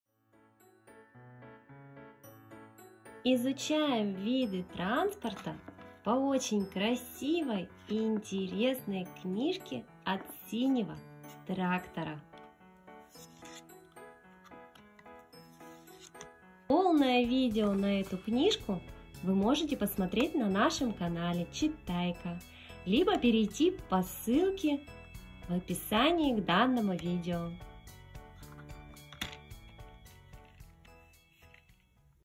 Обзор книжки для детей "Синий Трактор. Транспорт На дороге"
Литературно-художественное издание
Для чтения взрослыми детям 0+
Автор стихов Анна Купырина

Что Вас ждет под обложкой:
Встречайте Синий трактор и его друзей на страницах ярких книжек! Весёлые стишки и красочные развороты сделают чтение не только интересным, но и полезным!
Данная книга очень яркая и красочная. Детям интересно рассматривать яркие рисунки и выполнять задания.